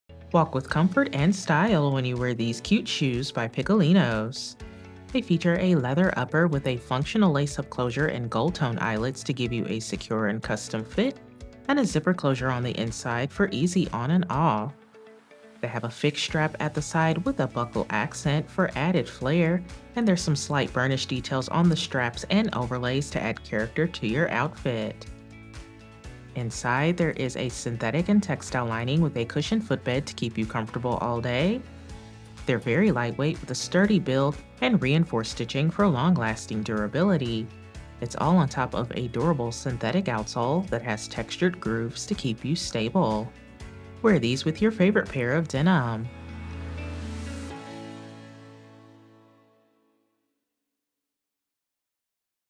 Pikolinos Lagos 901-7312C4 SKU: 9785047
Leather sourced from a tannery that achieved a Silver Rating from the Leather Working Group (LWG).
Walk with comfort and elegance wearing Pikolinos® Lagos 901-7312C4 Women's Shoes.
Leather upper.
Textile lining.
Cushioned insole.
Laces and zipper closure.
Decorative buckle.
Brand logo on the tongue.
Synthetic outsole.
Imported.

Product measurements were taken using size EU 39 (US Women's 8.5-9), width B - Medium. Please note that measurements may vary by size.